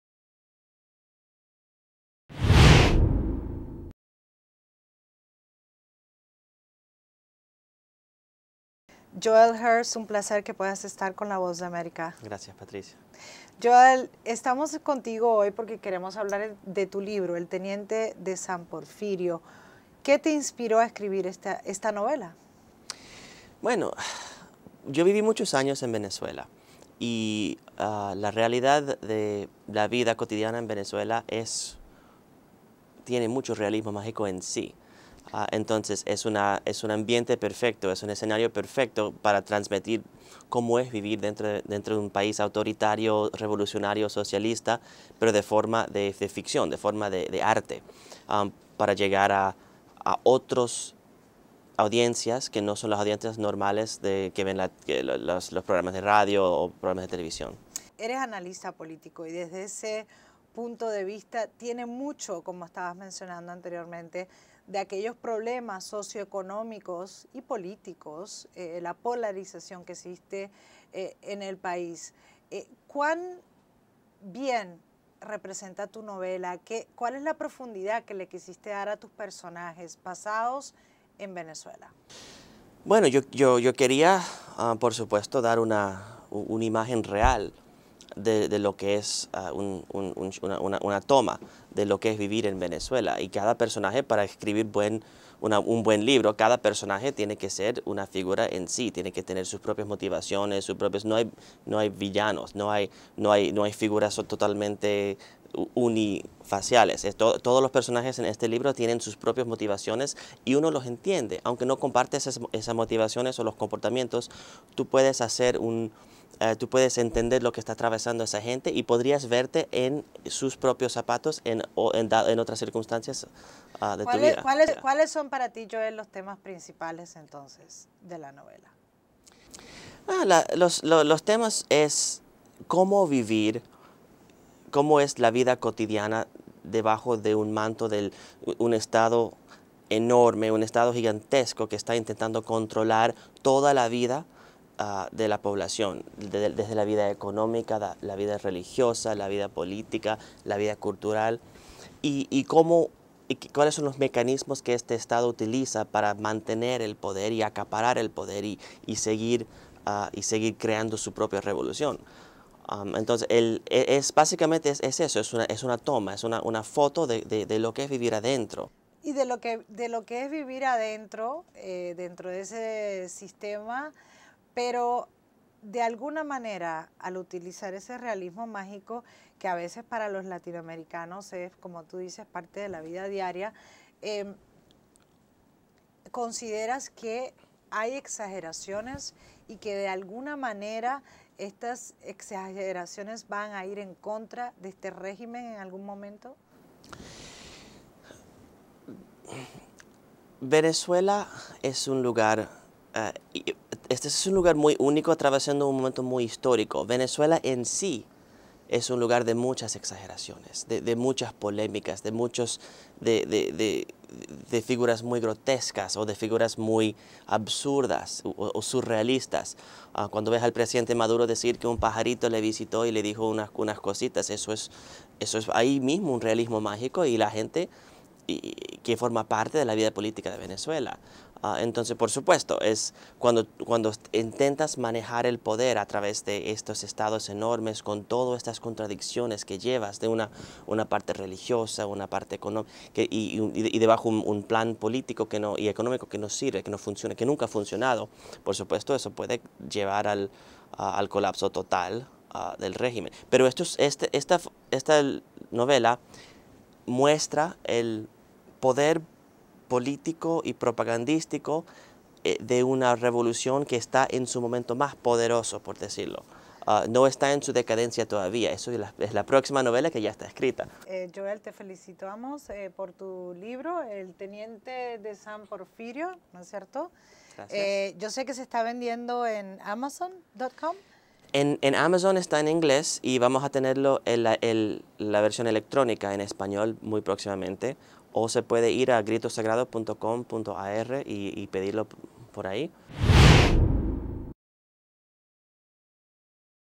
El teniente de San Porfirio
Entrevista a Joel Hirst, autor de la novela "El teniente de San Porfirio", conducida por Patricia Dalmasy.